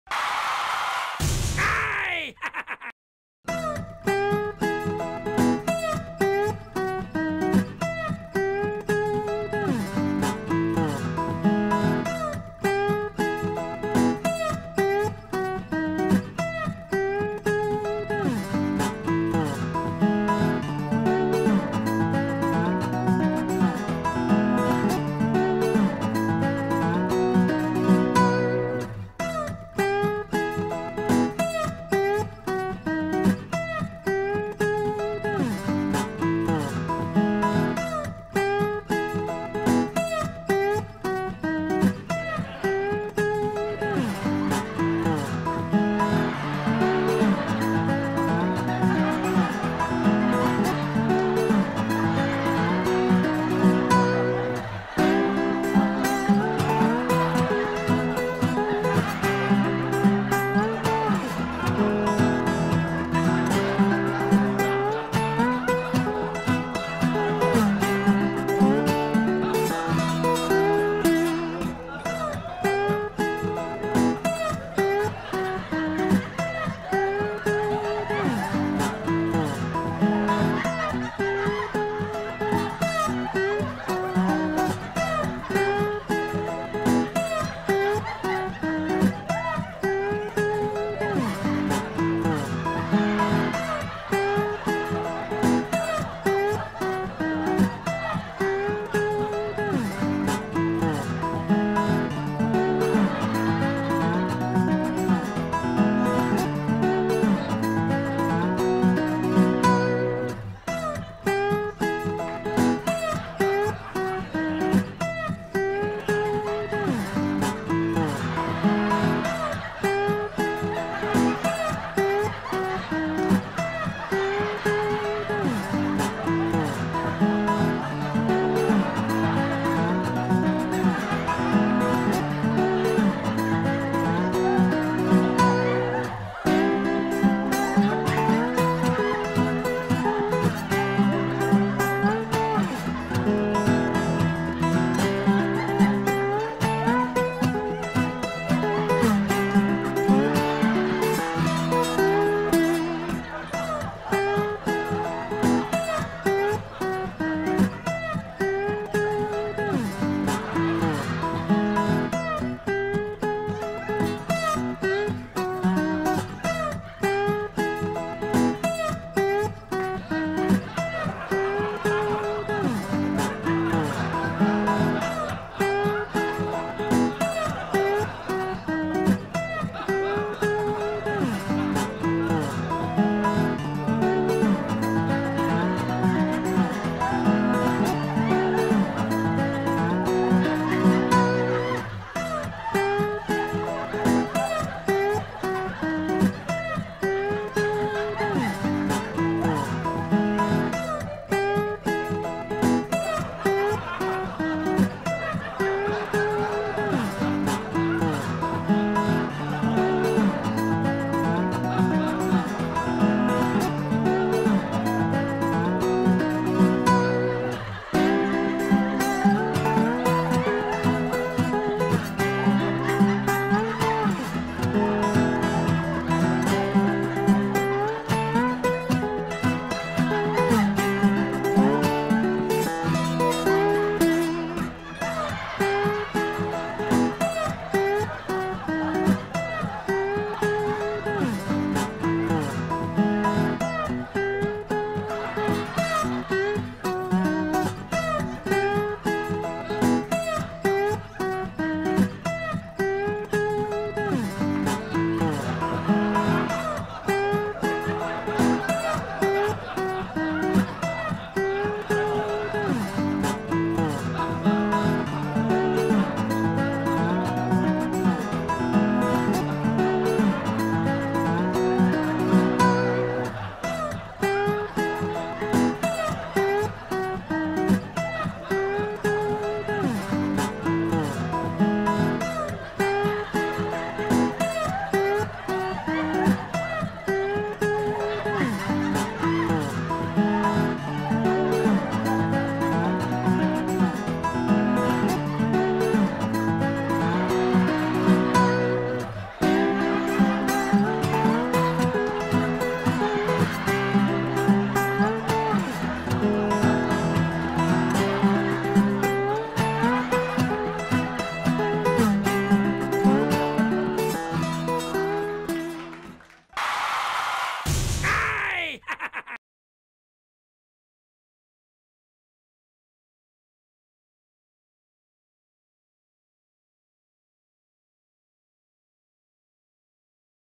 Using The Wrong Side of The Pen PRANK
Our actor disguised as an injured man frustrates strangers by using the wrong side of a pen